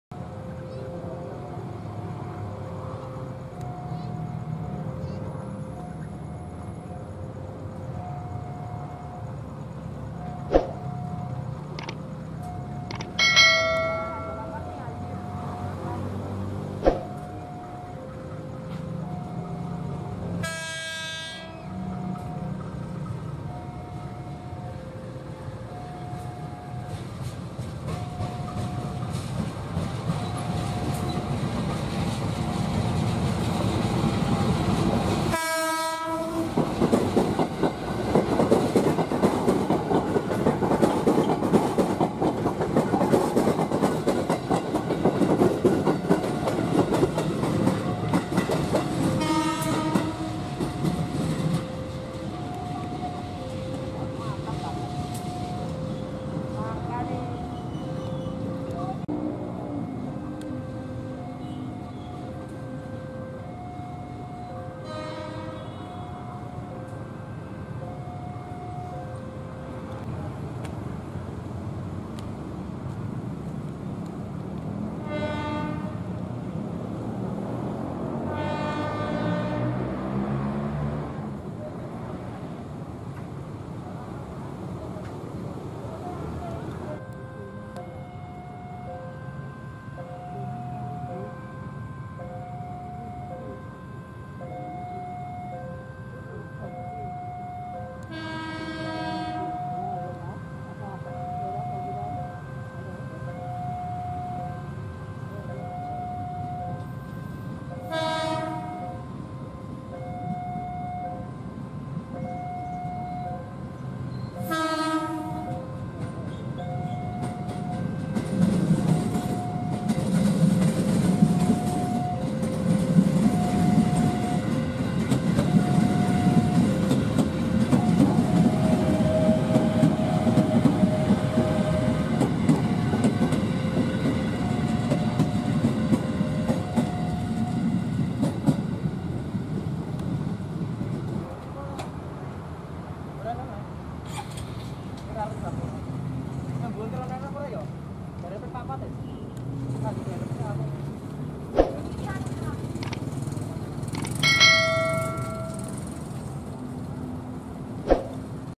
KRL JOGJA-SOLO | UJI COBA KRL JR205-9
Kereta Rel Listrik (KRL) JR 205-9 nantinya akan beroperasi di jalur KRL Yogya-Solo. Progres KRL Yogya-Solo saat ini tengah memasuki ujicoba sarana KRL.

Progres KRL Yogya-Solo saat ini tengah melakukan ujicoba lintasan dan sarana KRL dengan menggunakan KRL jenis KFW i9000 dan baru-baru ini akhirnya KRL JR 205-9 di ujicoba dari Stasiun Srowot Klaten menuju Stasiun Yogyakarta Tugu pulang pergi.

Moment Uji Coba ini saya dapatkan pada Tanggal 18 Des 2020, Selamat Menyaksikan :)